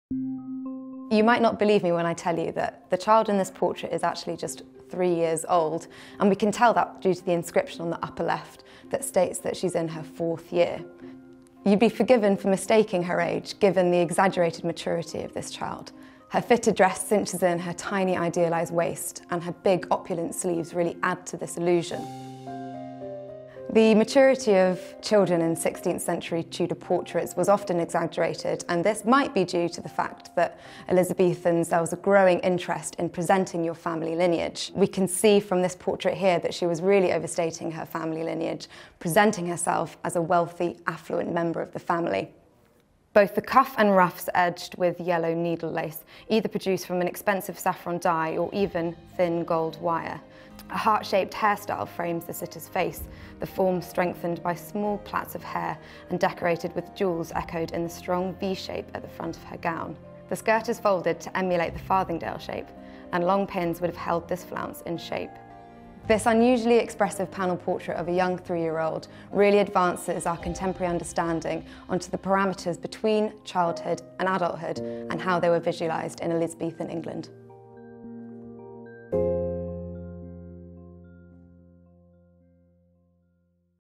Visualising Childhood in the Tudor Era
English sixteenth century portraits of children are rare, and when they were portrayed the child’s maturity was often exaggerated.

This overstated association with adulthood is apparent in this portrait of a young three-year-old girl.

Her maturity appears to have been exaggerated to the extent that her silhouette alludes to that of a grown woman; her intricately decorative, fitted gown which cinches in her narrow, idealised waist.